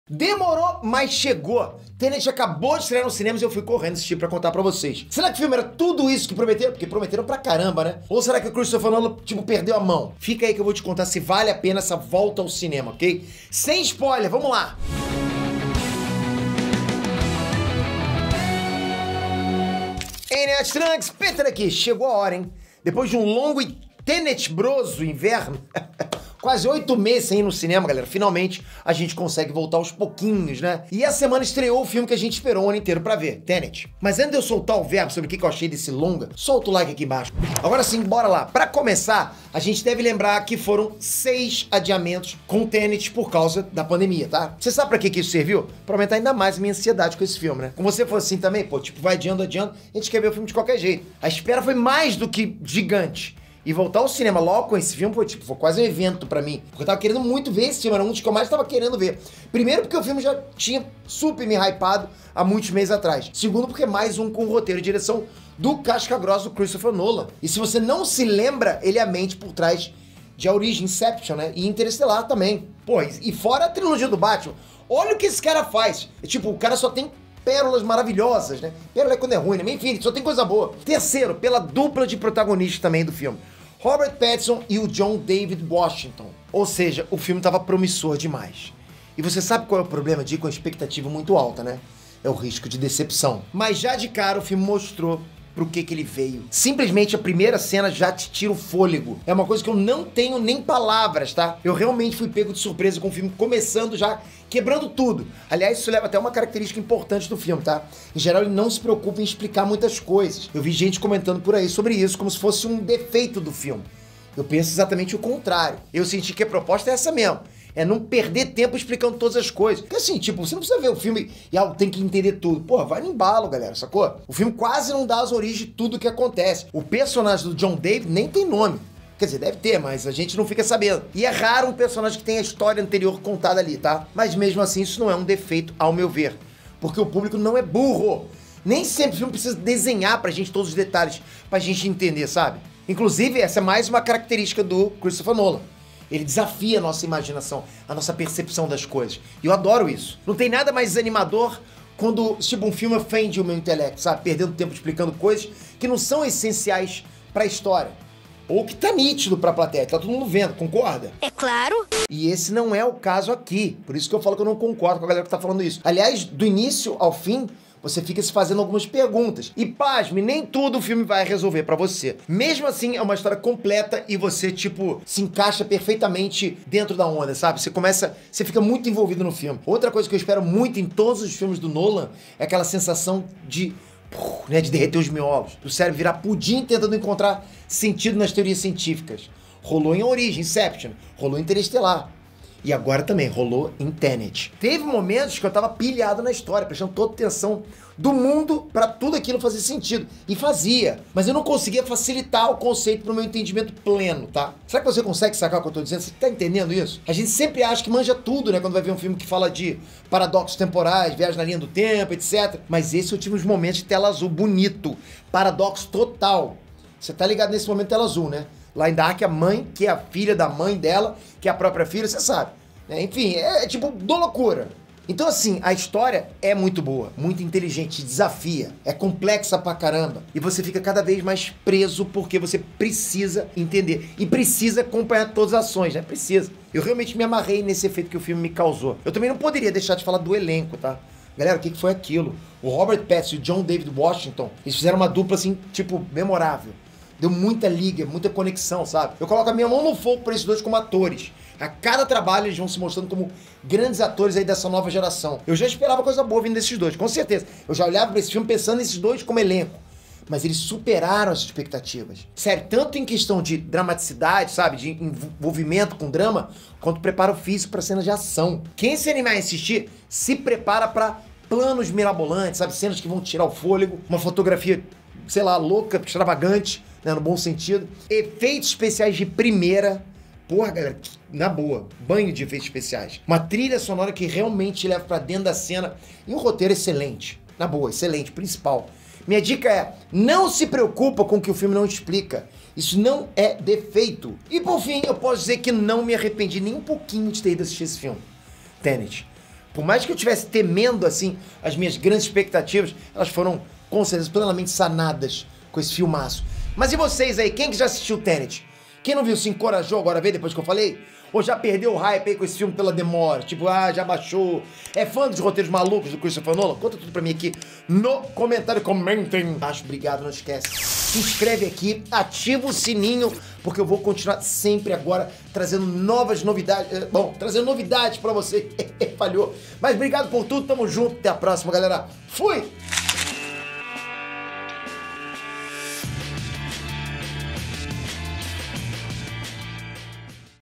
ASSISTI TENET - É BOM MESMO?
Demorou mas chegou! Tenet acabou de estrear nos cinemas e eu fui correndo assistir pra contar pra vocês! Será que o filme era isso tudo que prometeram? Ou será que o Christopher Nolan perdeu a mão? Fica aí que eu vou contar se vale a pena essa volta ao cinema! E sem spoiler!

💡 𝗔𝗽𝗼𝗶𝗲 𝗻𝗼𝘀𝘀𝗼 𝗰𝗮𝗻𝗮𝗹:
✓Baixe o app do Banco BS2 e abra a sua conta. É grátis

Petrópolis / RJ